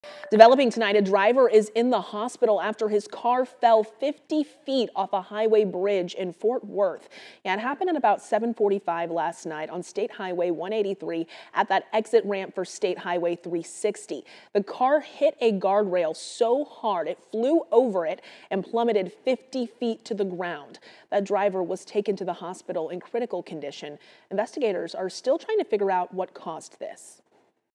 Car falls off Fort Worth highway bridge, driver hospitalized
Police said the car fell about 50 feet to the ground below.